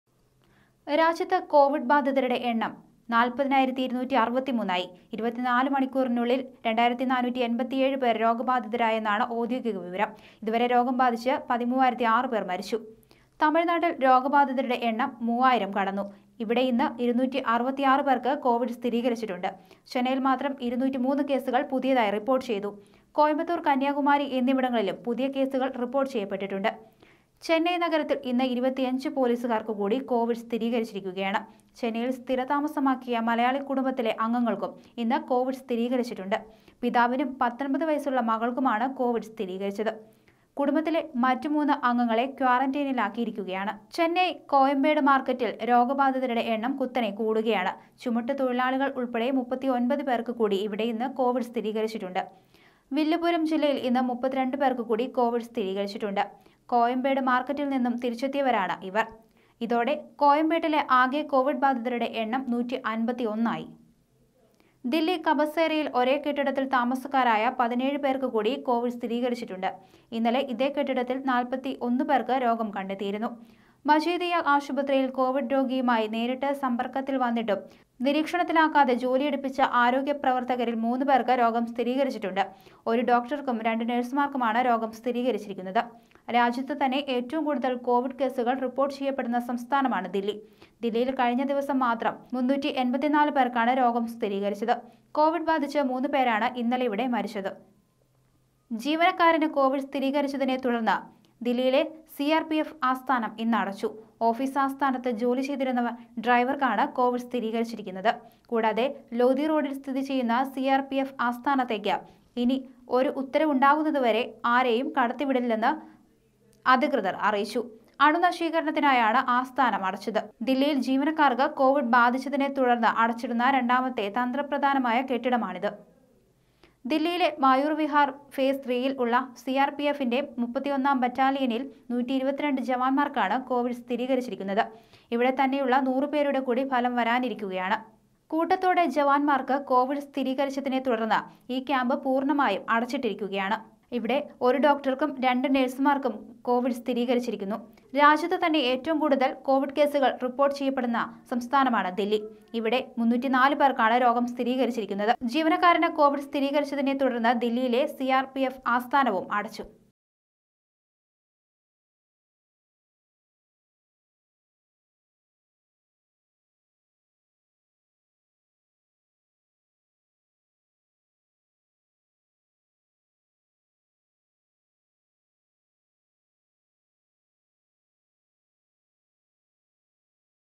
കൊവിഡ് ബാധിതര്‍ 40,000 ല്‍ അധികം l india
ചെന്നൈ കോയമ്പേട് മാര്‍ക്കറ്റില്‍ രോഗ ബാധിതരുടെ എണ്ണം കുത്തനെ കൂടുകയാണ്. ചുമട്ടുതൊഴിലാളികള്‍ ഉള്‍പ്പടെ 39 പേര്‍ക്ക് കൂടി ഇവിടെ ഇന്ന് കൊവിഡ് സ്ഥിരീകരിച്ചു. ആയി. വില്ലുപുരം ജില്ലയില്‍ ഇന്ന് 32 പേര്‍ക്ക് കൂടി കൊവിഡ് സ്ഥിരീകരിച്ചിട്ടുണ്ട്. കോയമ്പേട് മാര്‍ക്കറ്റില്‍ നിന്നും തിരിച്ചെത്തിയവരാണ് ഇവര്‍. ഇതോടെ കോയമ്പേട്ടിലെ ആകെ കൊവിഡ് ബാധിതരുടെ എണ്ണം 151 ആയി